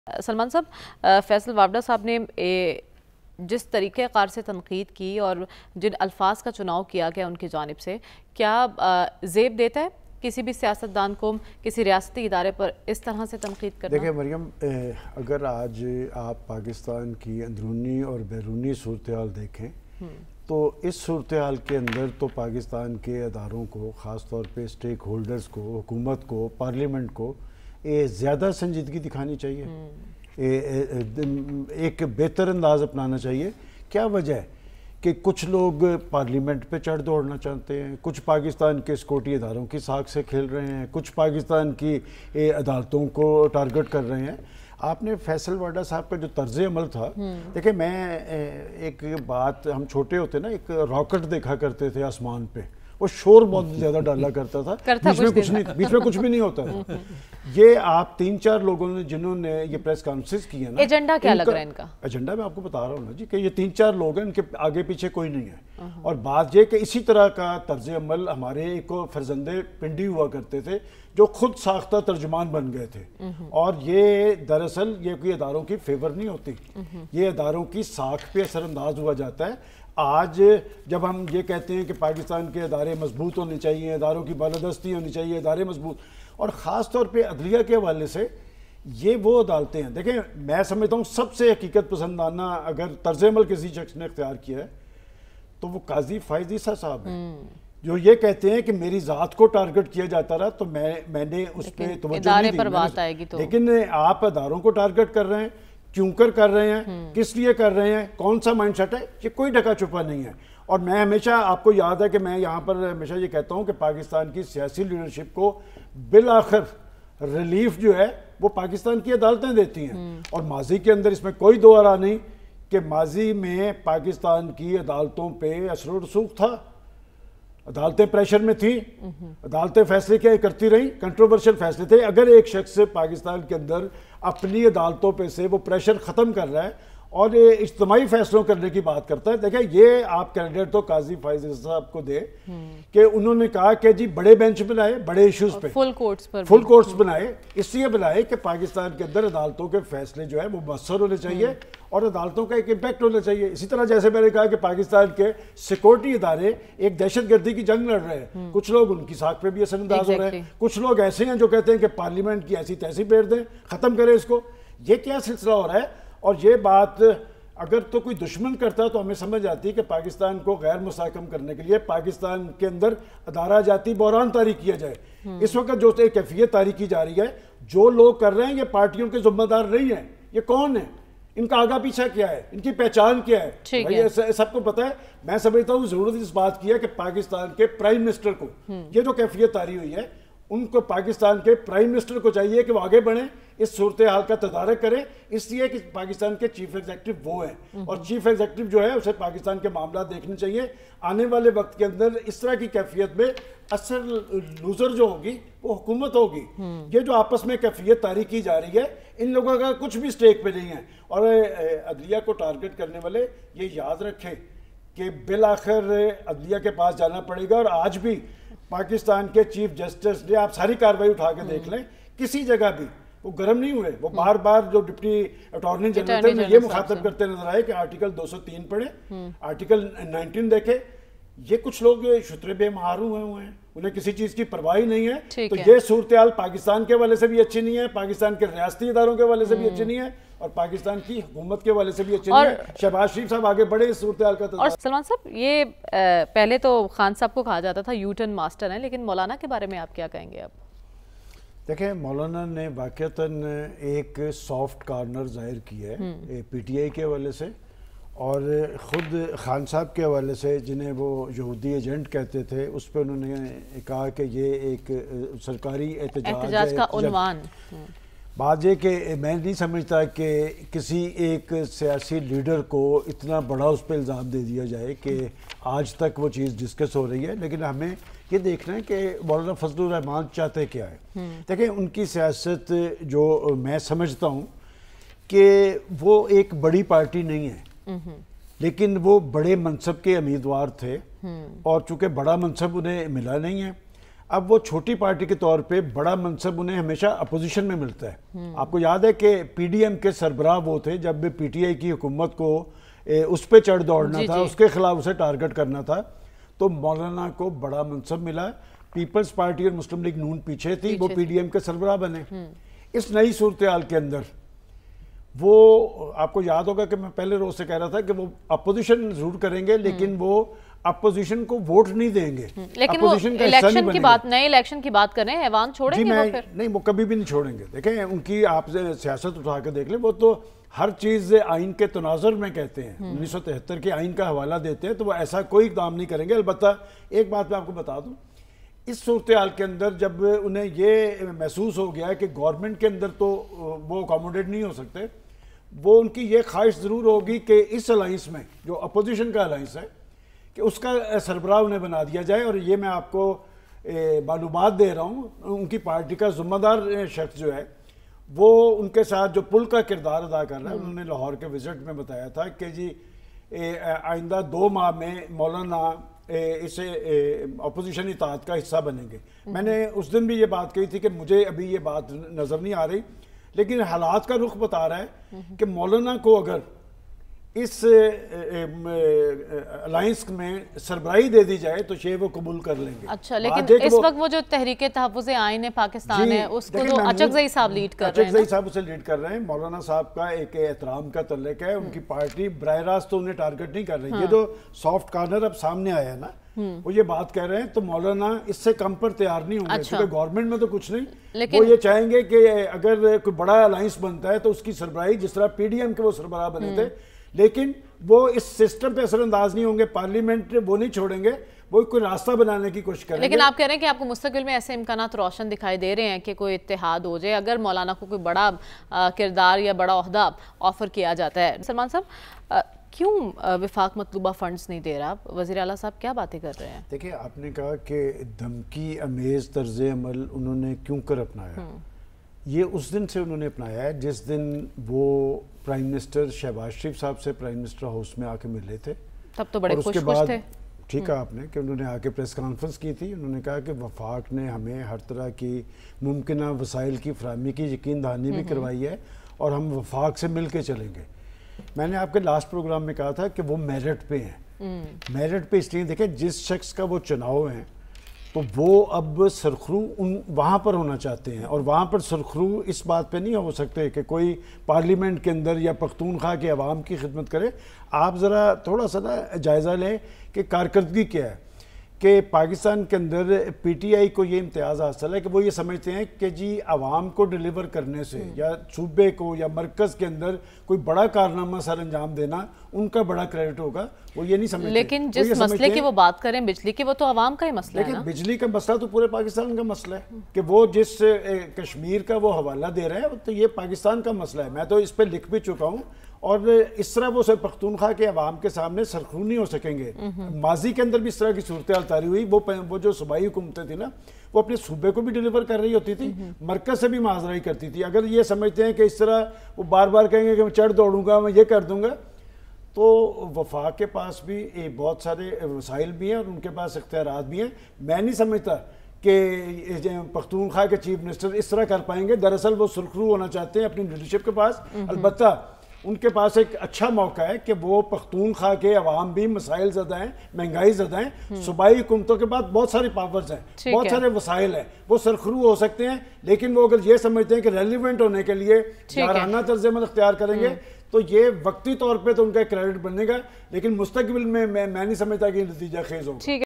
Who Is The Mastermind? | Salman Ghani Great Analysis | Think Tank
Discussion on the current political and economic scenario: Debate Among Anchorperson Marrium Zeeshan  Rasool Baksh Raees Rasheed Safi Hasan Askari and Salman Ghani.